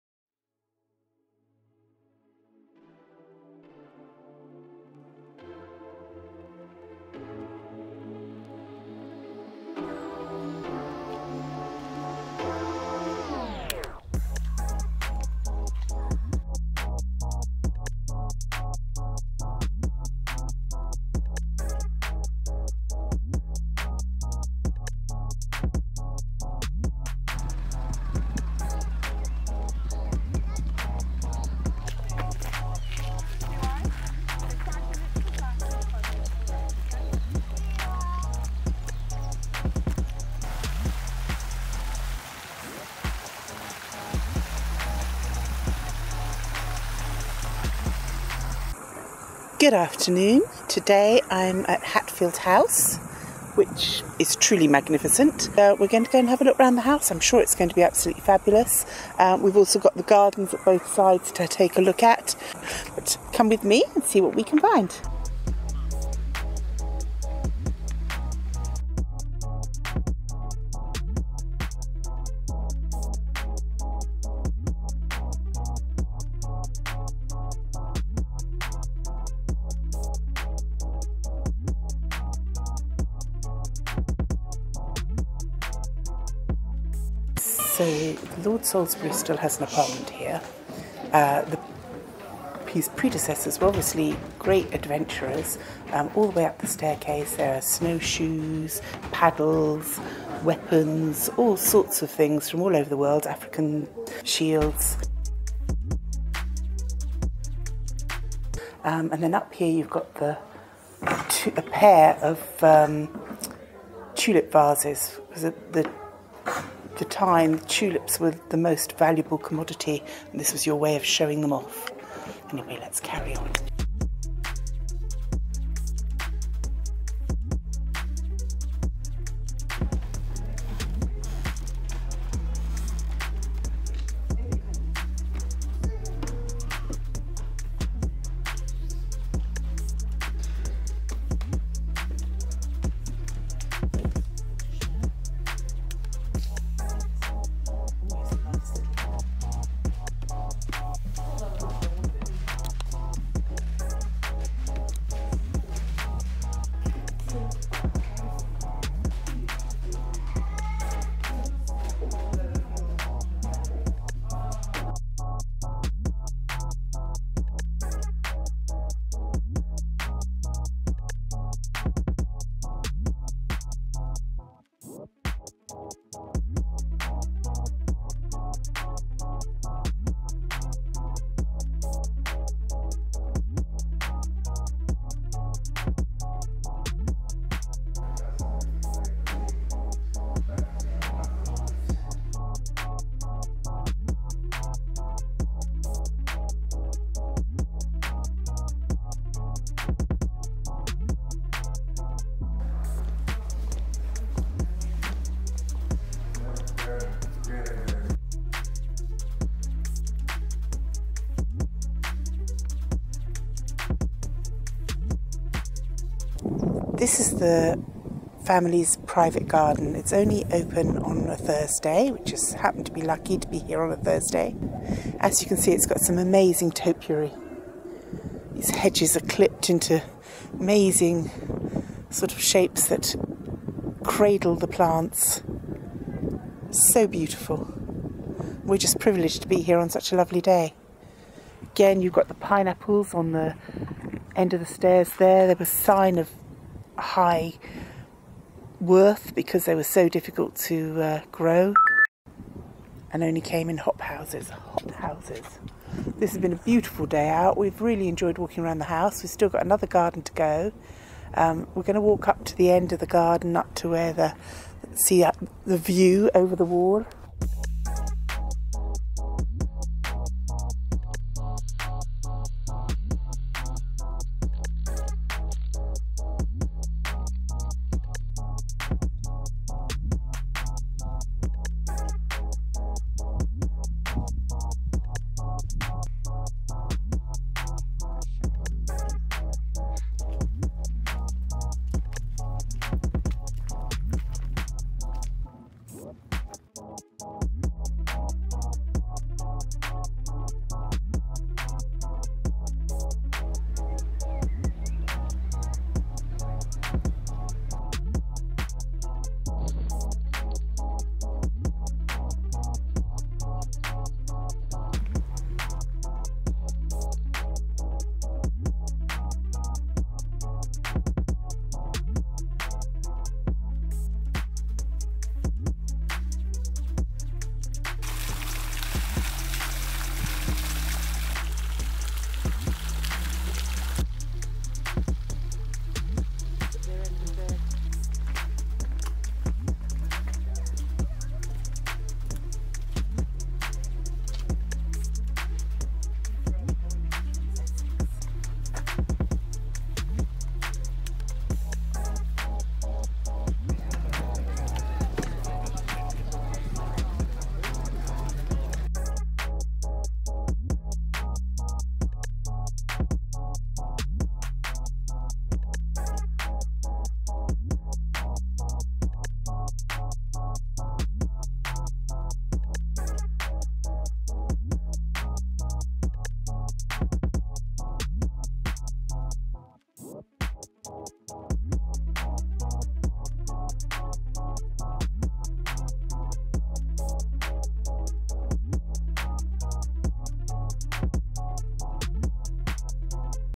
Hatfield House (bonus vlog)
Hatfield House is one of the grand houses and gardens of the UK and well worth a visit.
Come with me and have a quick look.